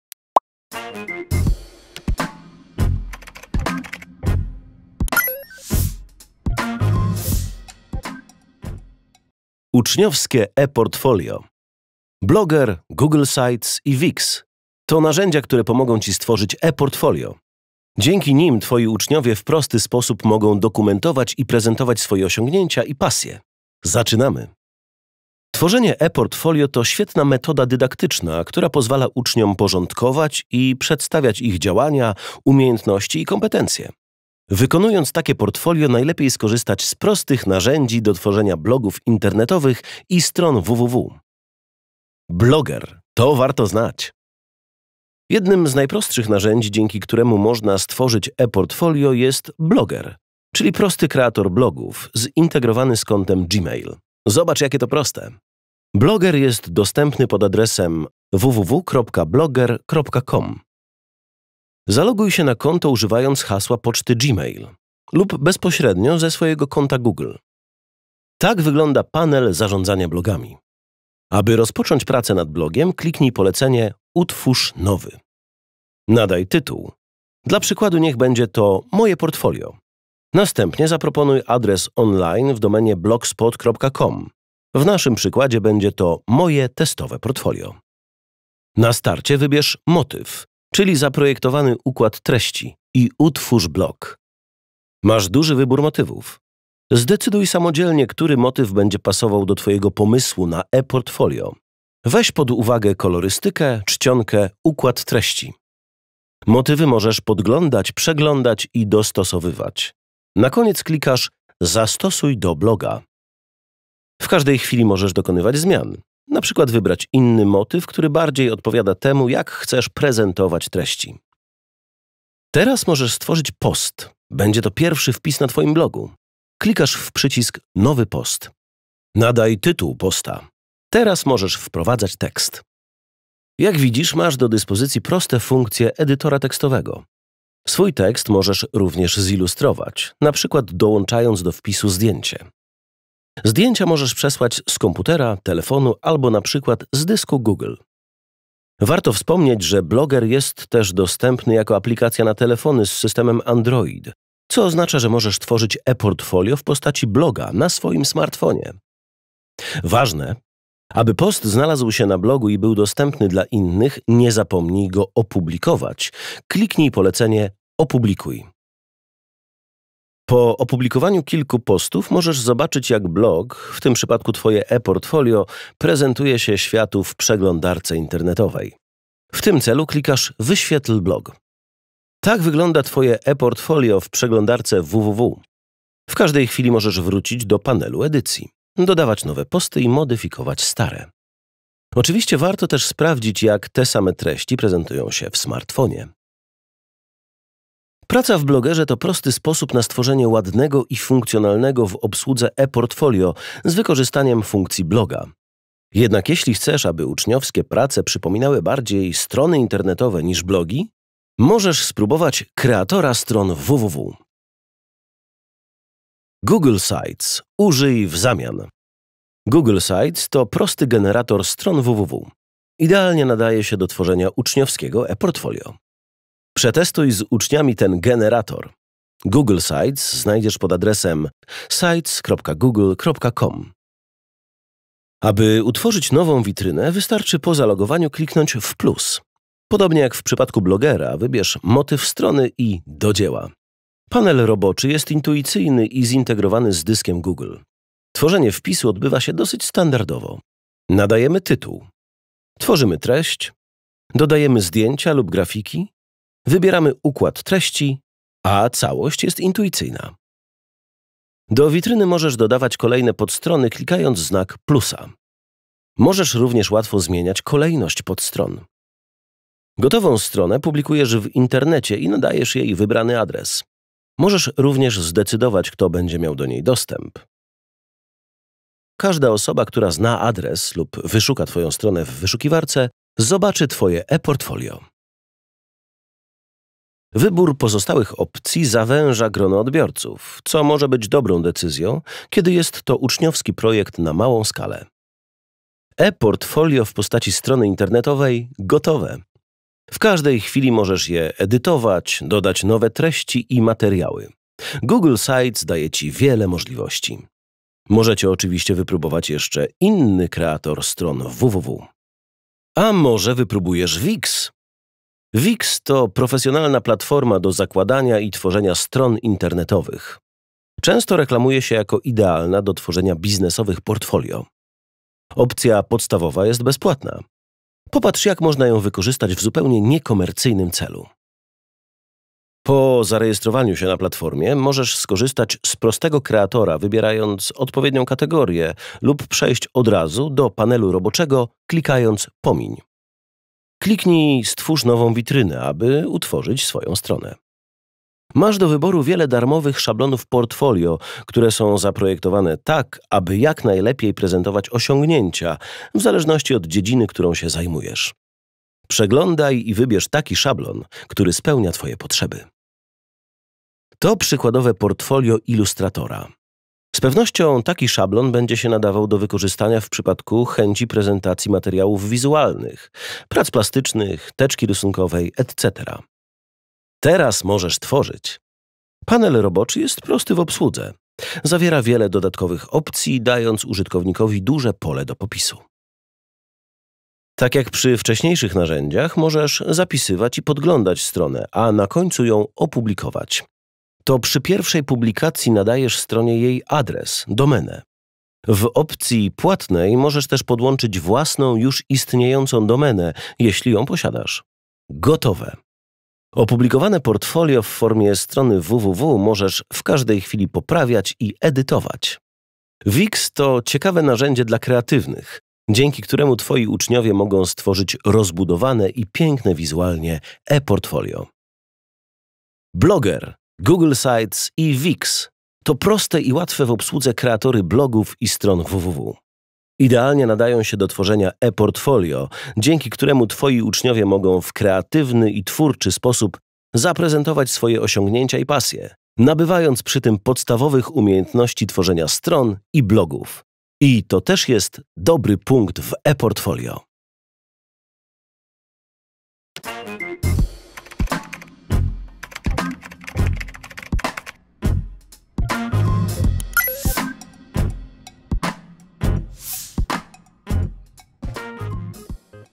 Uczniowskie e-portfolio
Tworzenie e-portfolio to świetna metoda dydaktyczna, która pozwala uczniom porządkować i przedstawiać ich działania, umiejętności i  kompetencje. Wykonując takie portfolio, najlepiej skorzystać z prostych narzędzi do tworzenia blogów internetowych i stron www. Dowiedz się, jak działają trzy narzędzia, które pomogą stworzyć e-portfolio Tobie i Twoim uczniom:
- Blogger – to warto znać
- Google Sites (Witryny Google)– użyj w zamian
- A może spróbujesz WIX?
Autorka scenariusza: Izabela Wyppich.
Materiał powstał w ramach projektu "Lekcja:Enter", współfinansowanego ze środków Unii Europejskiej w ramach Programu Operacyjnego Polska Cyfrowa. Więcej informacji